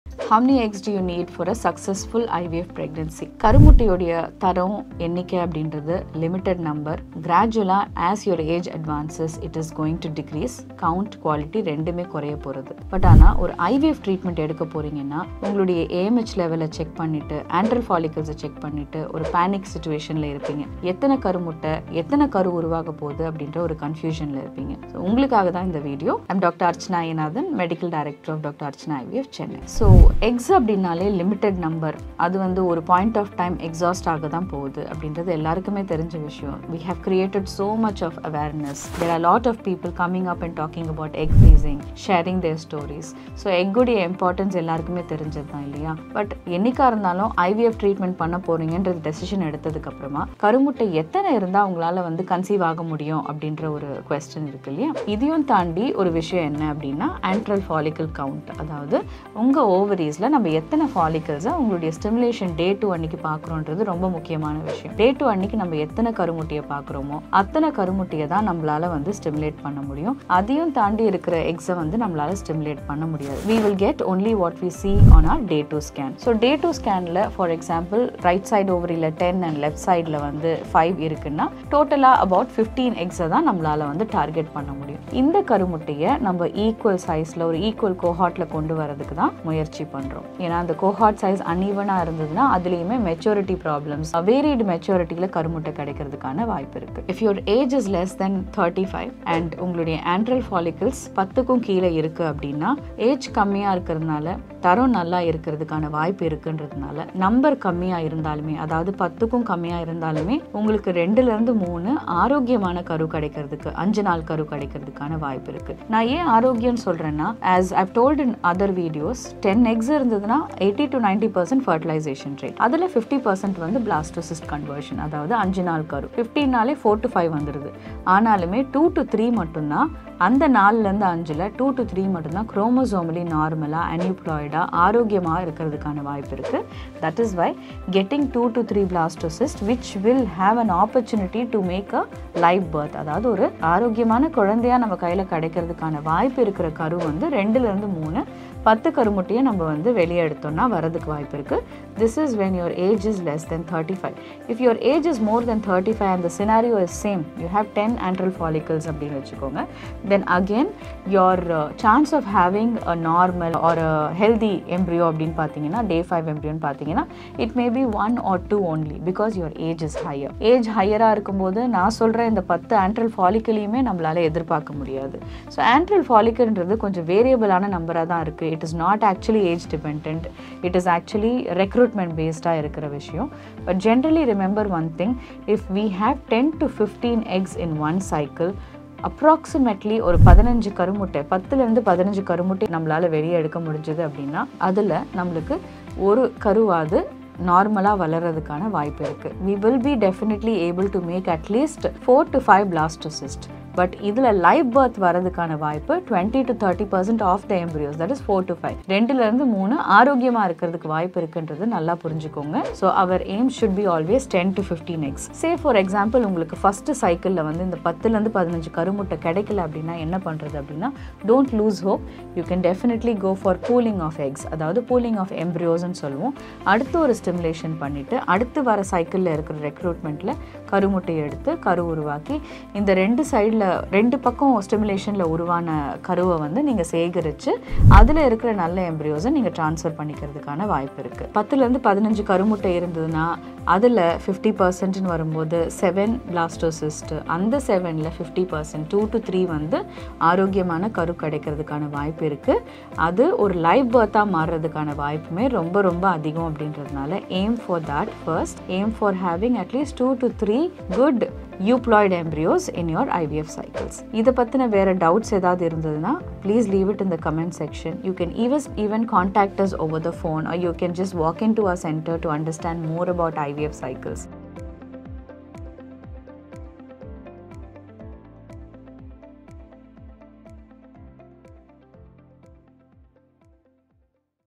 How Many Eggs Do You Need For A Successful IVF Treatment? | - Dr. Archana S Ayyanathan
How Many Eggs Do You Need For A Successful IVF Treatment?

For more updates on pregnancy and women's health

follow Dr. Archana S Ayyanathan